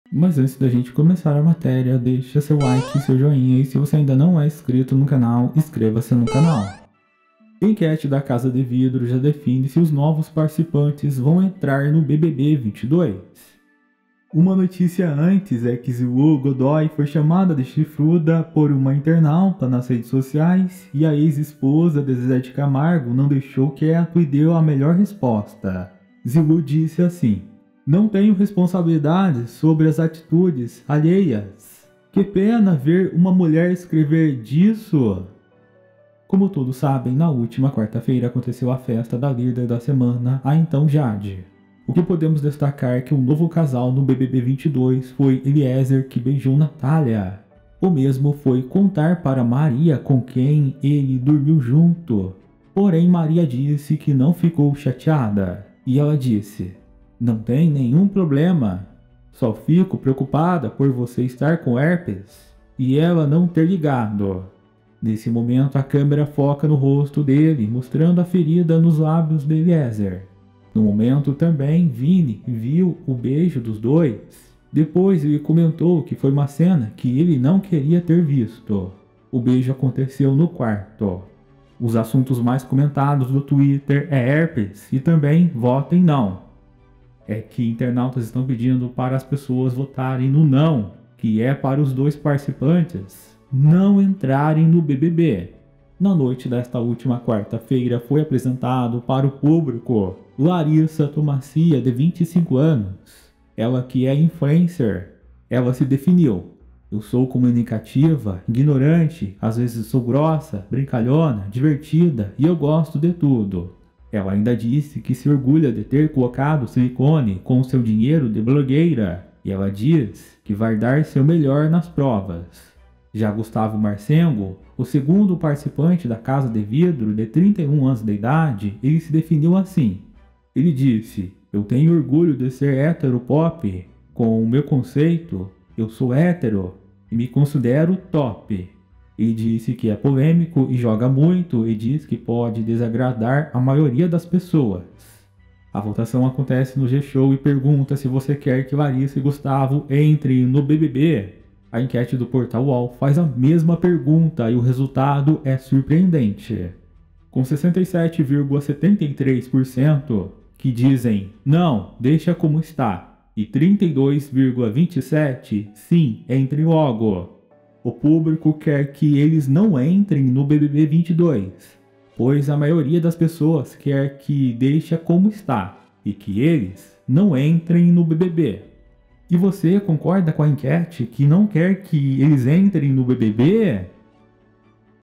🔴 Enquete da Casa de Vidro, já define se Novos Participantes vão Entrar no BBB22
Enquete da Casa de Vidro, já define se Novos Participantes vão Entrar no BBB22. após a festa, casa de vidro.